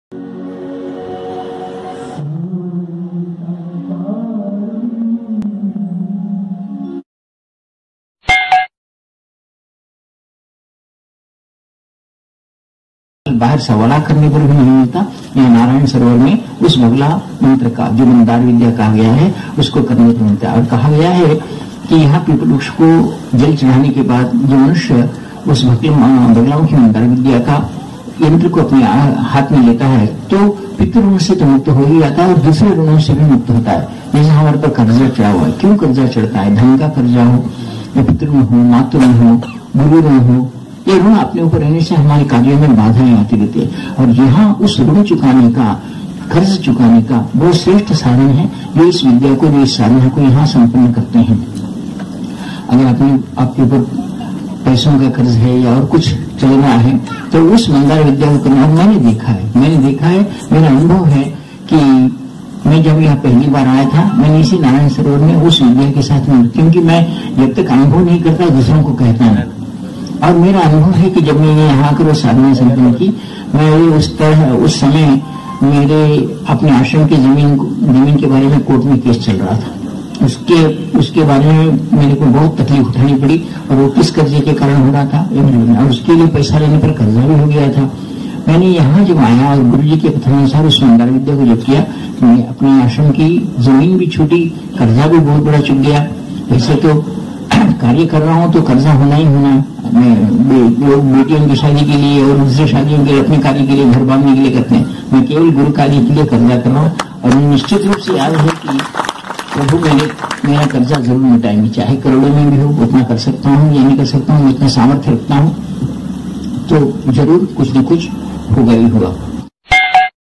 रूणामुक्ति गुरूदेव का अनुभव बगलामुखी साधनाGurudev experience,..... Bagalamukhi sadhana.
Bagalamukhi Sadhana performed by gurudev and he relieved from debts.In this video listen speech of Gurudev.